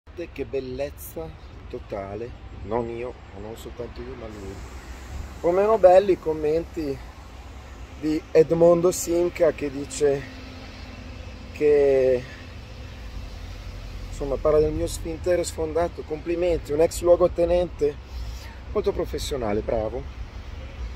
127 RUBATA / diffamazioni dall'ex carabiniere
127 RUBATA / diffamazioni dall'ex carabiniere

continuano gli entusiasmanti ed edificanti commenti da parte dell'autore del furto della Fiat 127 del 1971, targata BS 365 087, così coraggioso da continuare a nascondersi dietro ai suoi profili finti principali: "Edmondo Simca" ed "Eva Kent".

Non è uno scherzo: si tratta veramente di un ex carabiniere, tanto da essersi presentato in divisa, quando lo vidi per la seconda volta e mi disse di voler avviare un programma di protezione, per gli attacchi e le minacce che ho subito da Ottavio Chiodini, Mattia Miorali e soci.

Peccato fosse d'accordo con loro.

Peccato stia scrivendo, ovunque, che io sia un cul***one, un eb**o, un figlio di NN (lo ha detto Daniele Parietti, quindi è vero), un truffatore, uno schilometratore (lo ha detto Boren Rossi, quindi sarà proprio vero) etc etc.

Beh, qualcuno avrà pur staccato le targhe alla Delta, che da oggi peraltro metterò in vendita, perché mi sto veramente stomacando di tutto questo...

Mentre è certo che qualcuno mi abbia chiesto 70mila Euro, e che quel qualcuno sia proprio l'ex luogotenente, già denunciato.

EB / @htrreport